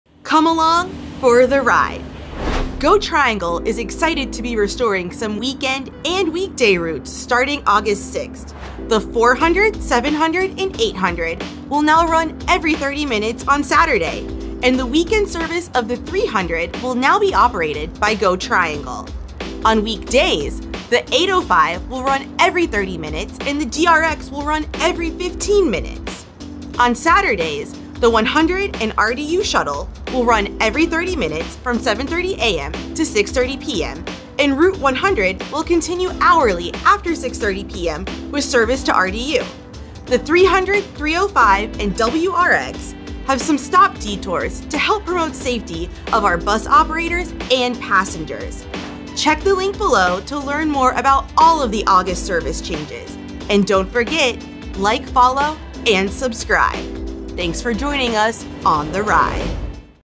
The Ride: GoTriangle Service Changes
Great news for your commute! August 6 service changes are here!
Restoration of service of weekend routes 100, 300, 400, 700 & 800 and weekday routes 805 and DRX! 🚌